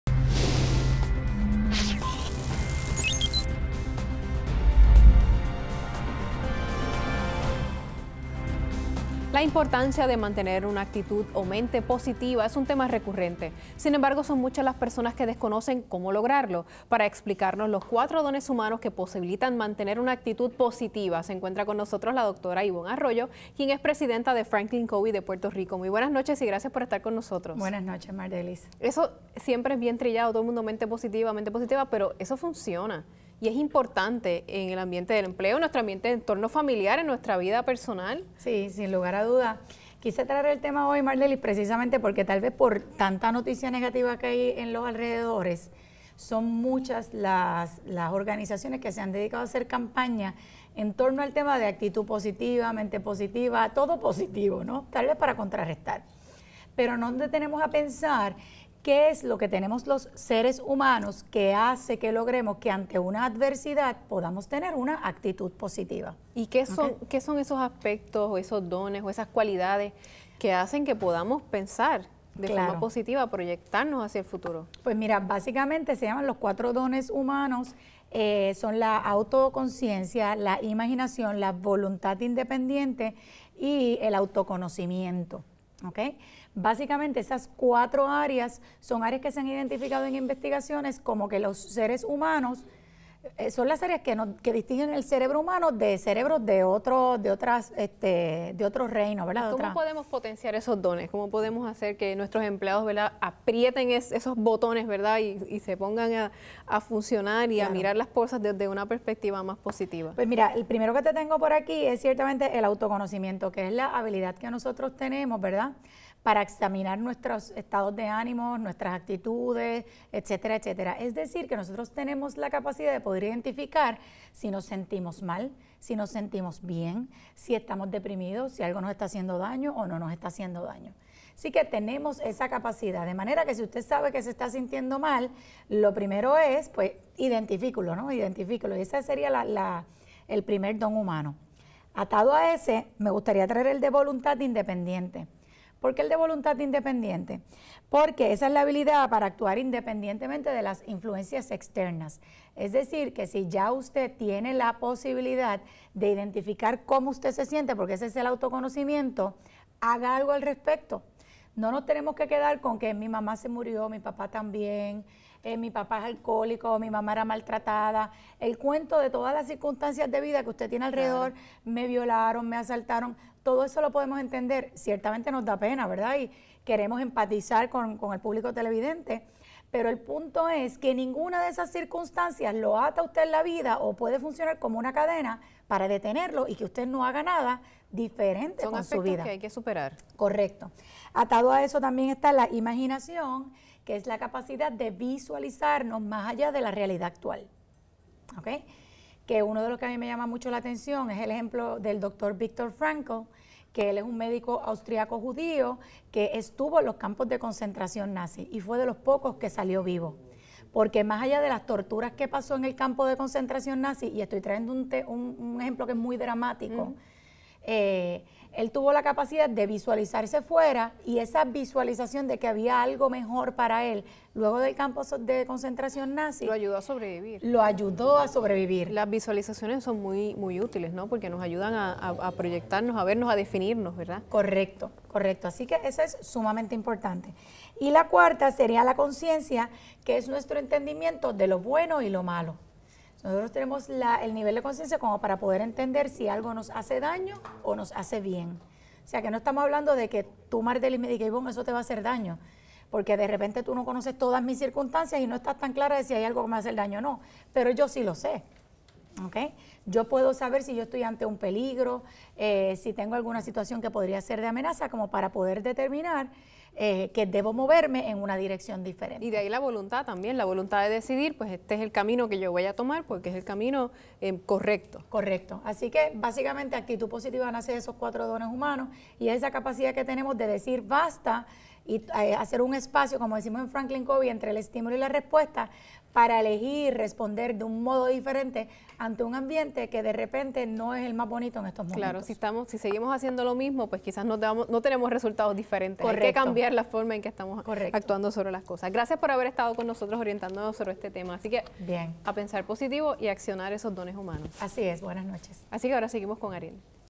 Sistema TV Informa: Desarrollo Organizacional - Mente y actitud positiva
La periodista Mardelis Jusino conversa con Ivonne Arroyo sobre cómo lograr ante una adversidad tener una actitud positiva. La entrevista presenta la fuente en "Los cuatro dones humanos", autoconocimiento, la conciencia, la voluntad independiente, y la imaginación creativa, explicando también como desarrollarlos.